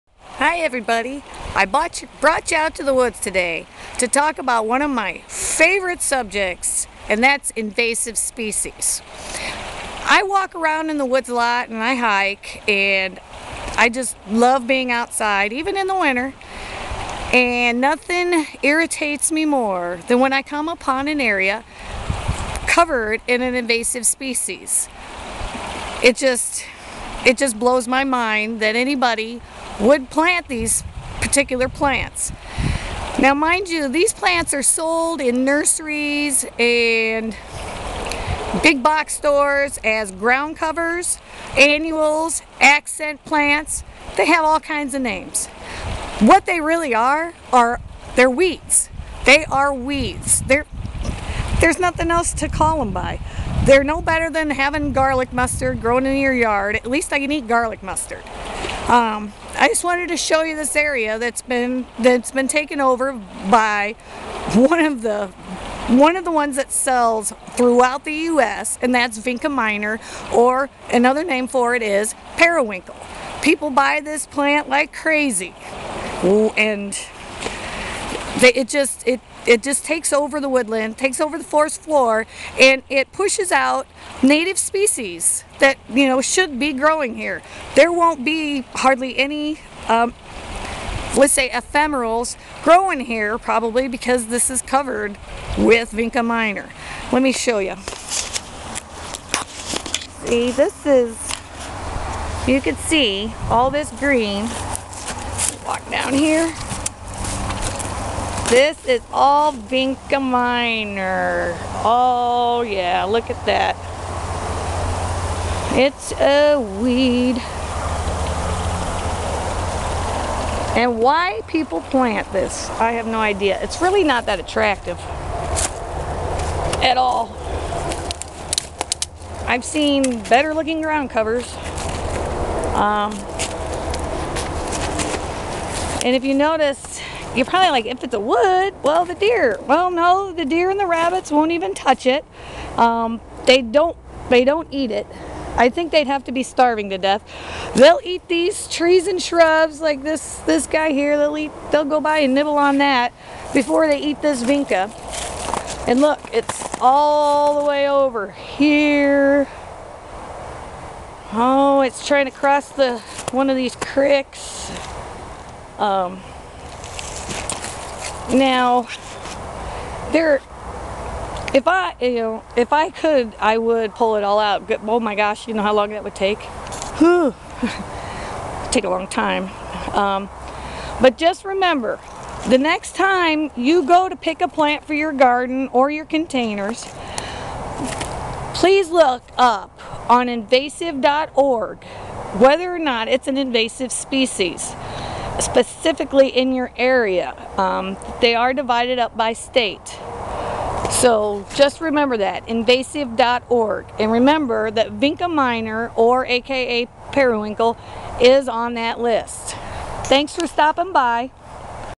Invasive Vinca minor aka Periwinkle
A short video that looks at invasive plant Vinca minor taking over a part of a woodland in Iowa.  Invasive species should not be sold/planted/introduced into our gardens because they can spread into wild areas.